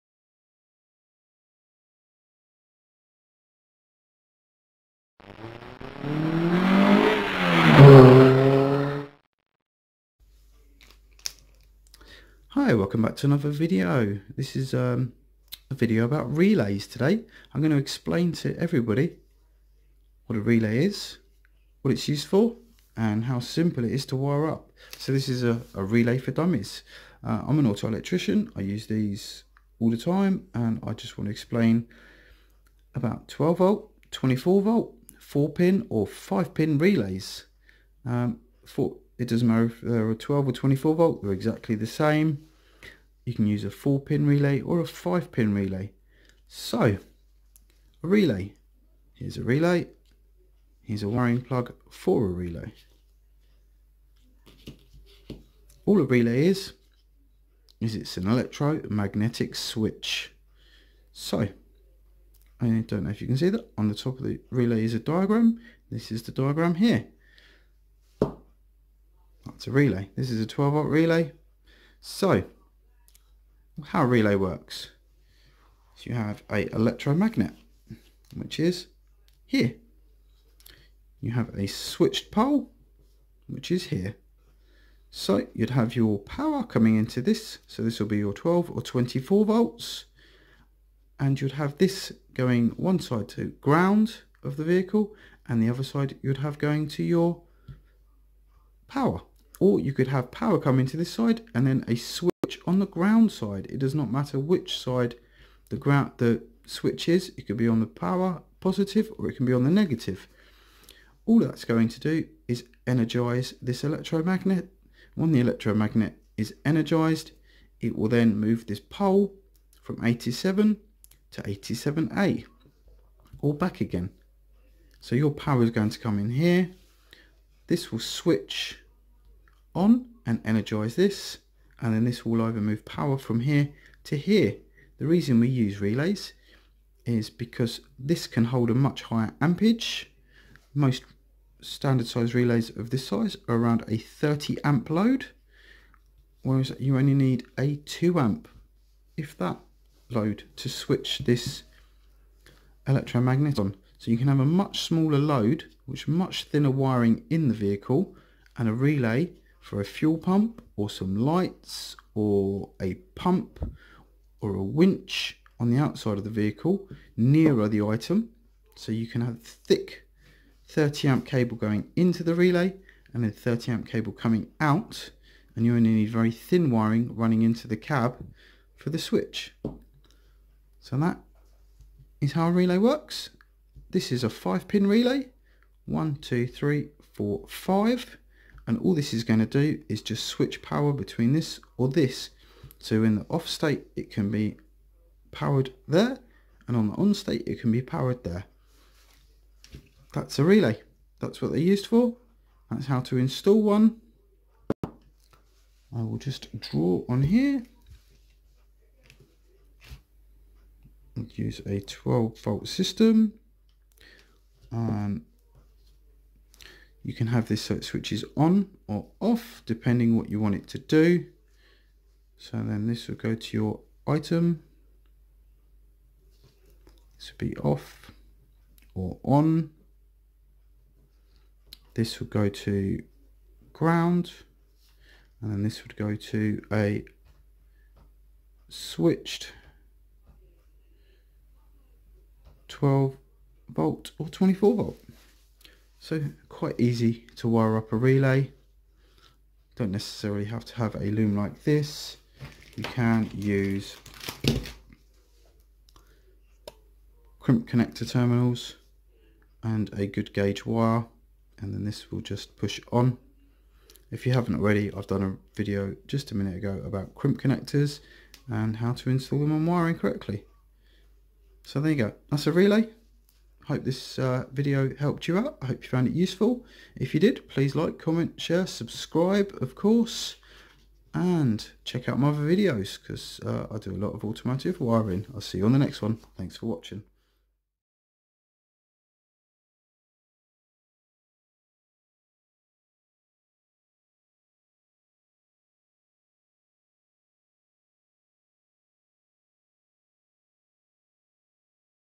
24v 12v relay explained 4 or 5 pin how to fit and how to use it
How to use a relay 12volt 24volt 4 pin or 5 pin how to use a relay and how to wire a relay and how a relay works